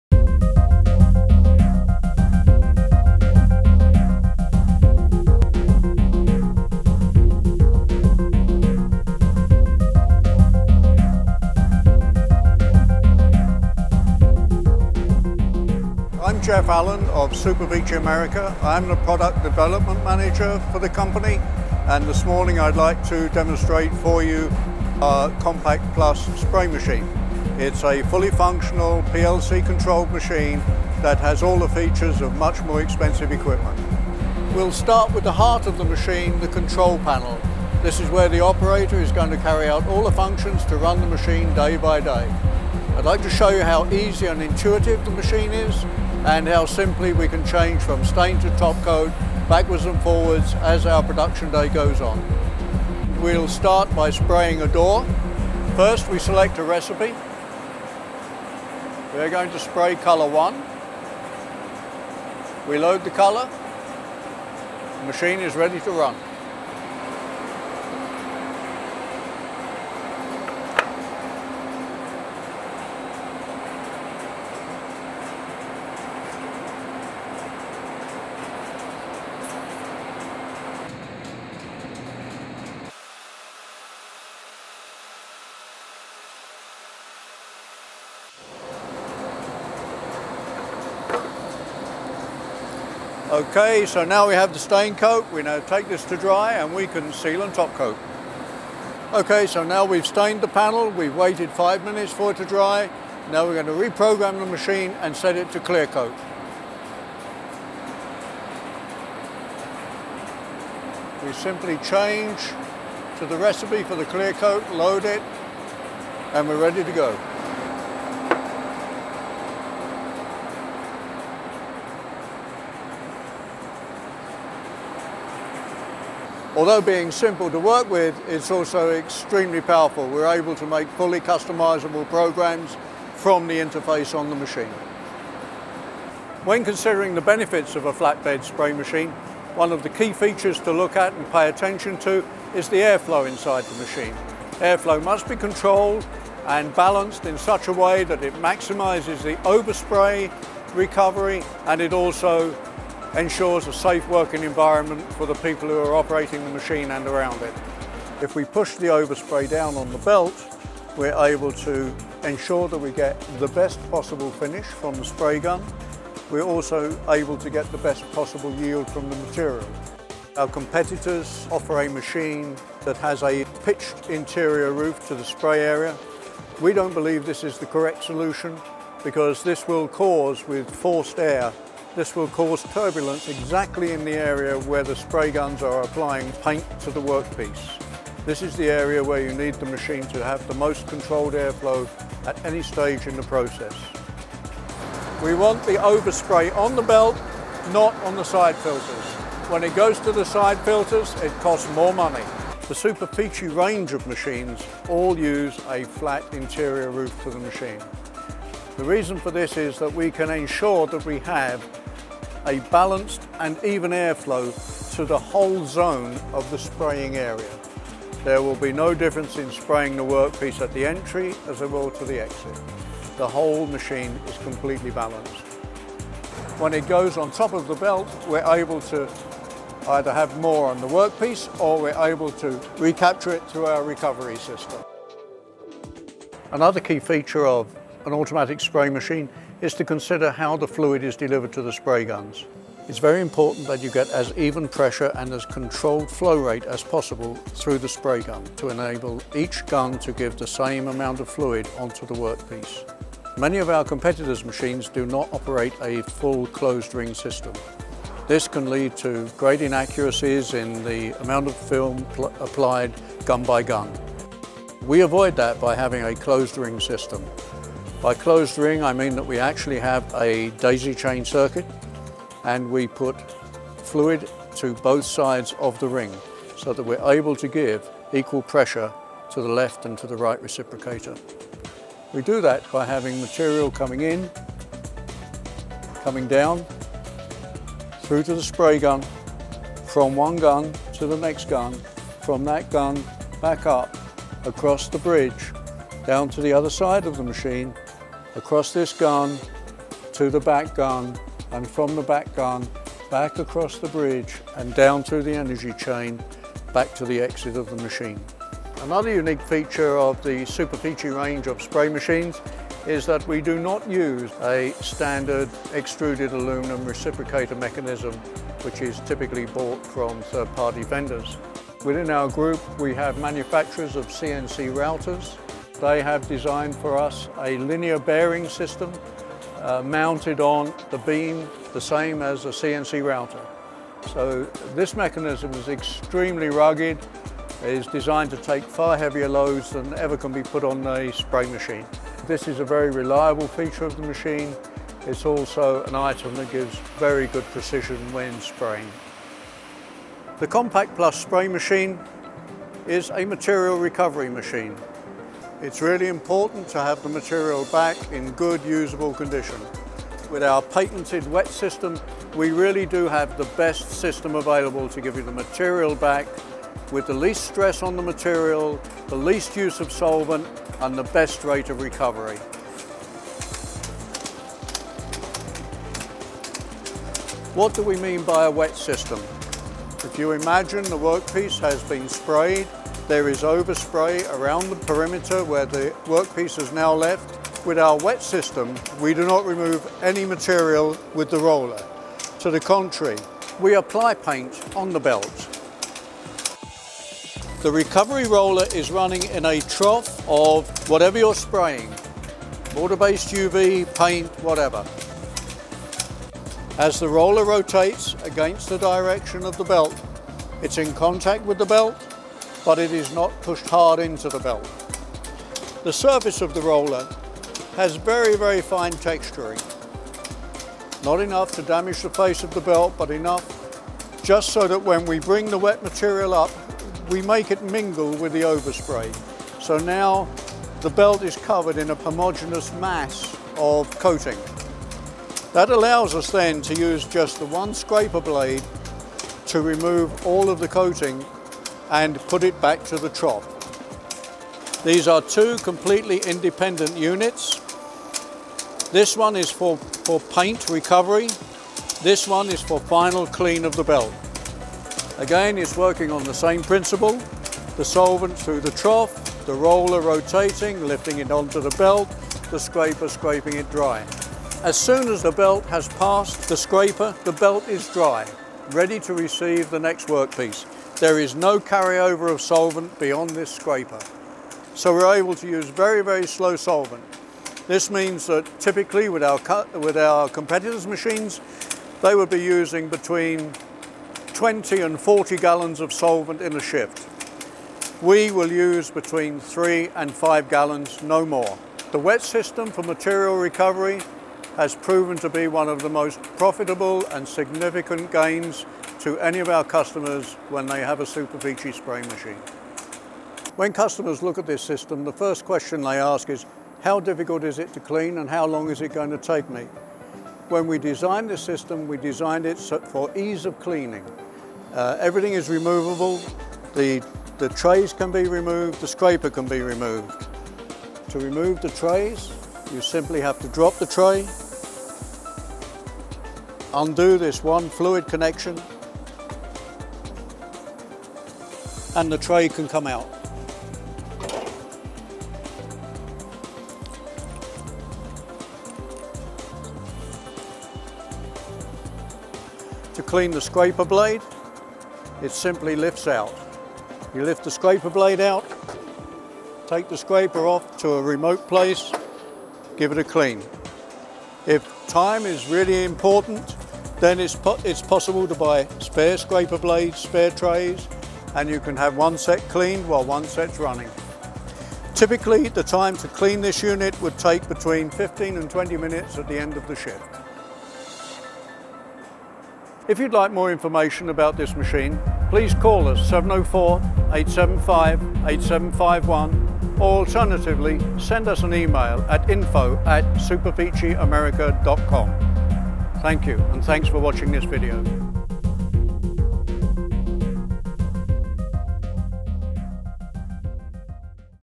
Superfici Compact Plus Spray Machine by SCM Group.mov
Sistema de aplicación del barniz con reciprocador de doble brazo accionado mediante motor brushless y montado en guía de precisión.
En cada uno de las dos brazos del reciprocador se puede aplicar hasta un máximo de cuatro pistolas, con dos circuitos (con la posibilidad de instalar un tercero circuito).
Dependiendo del tipo de producto y del peso que deben aplicarse, puede utilizar tres tipos diferentes de pistolas(no incluido en la entrega):
- Pistolas a baja presión
- Pistolas airmix
- Pistolas airless